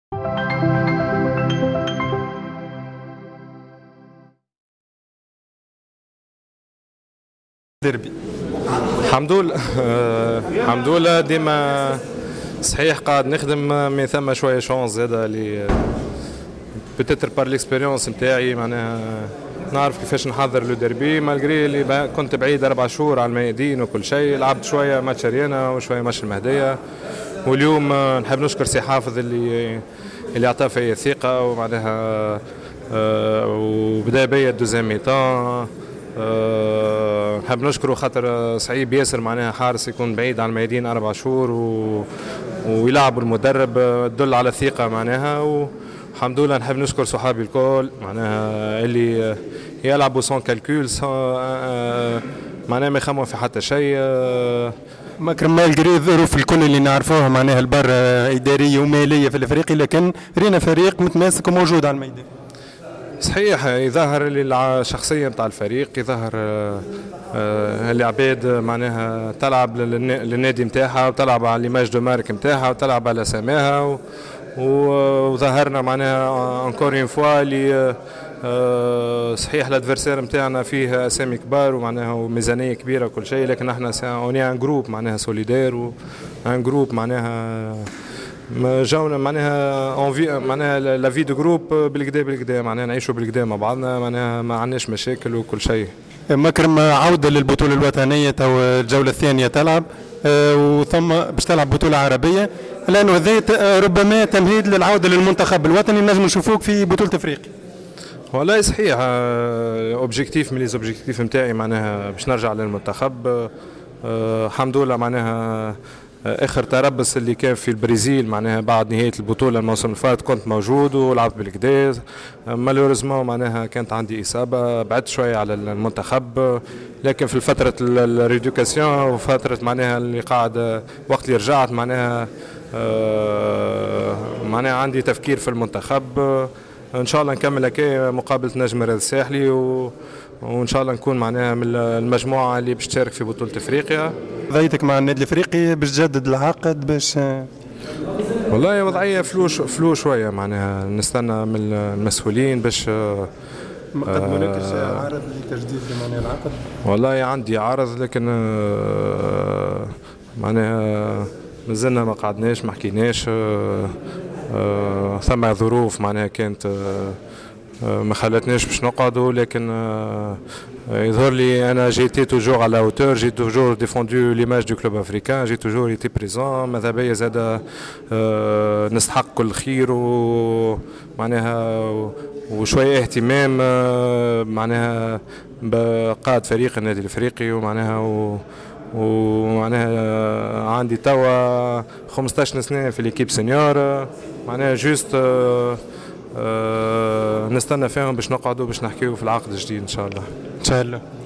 مكرم الميساوي.. وضعيتي صعبة في الأفريقي و هذا ما أطلبه من المسؤولين قبل فوات الأوان
بالفيديو..مكرم الميساوي.. وضعيتي صعبة في الأفريقي و هذا ما أطلبه من المسؤولين قبل فوات الأوان